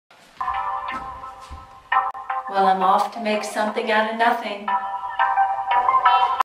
Something Out Of Nothing
Don't you think that this whole social media, especially You Tube, is something out of nothing?

These are some other popular video sharing apps:
YouTube. YouTube is an obvious and popular choice. ...
JW Player. ...
Wistia. ...
Vimeo. ...
Vidyard. ...
Loom. ...
Brightcove.

Well, I’m off to make something out of nothing.